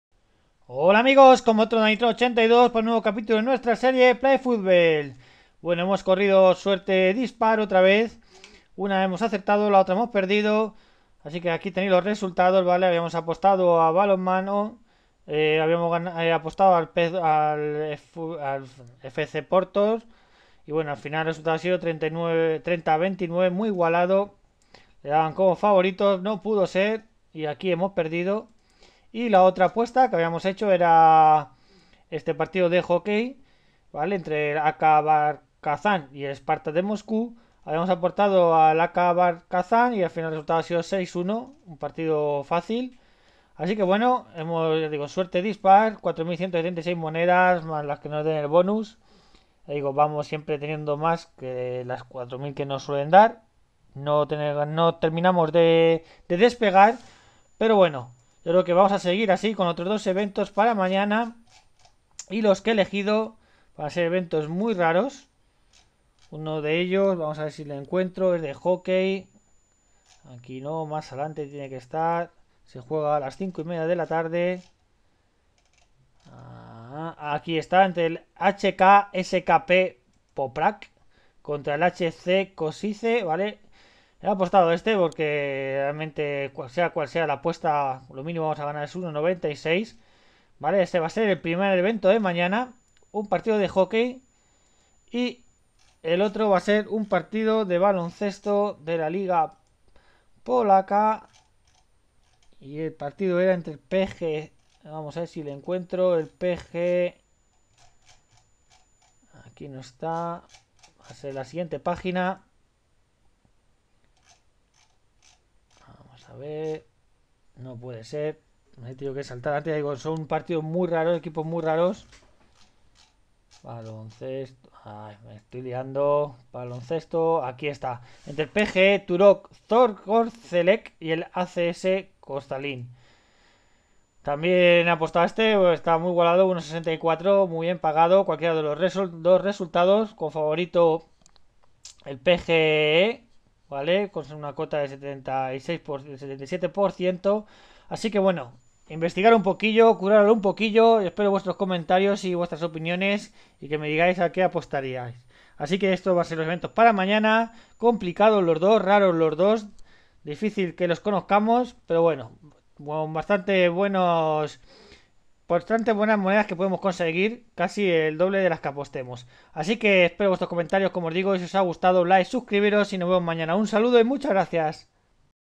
A medio ritmo!!! Playfulbet Ep30
Suerte dispar...uno si pero el otro fracaso. Aun asi con suficientes monedas para otros dos eventos interesantes mañana.

Saludos @danitron82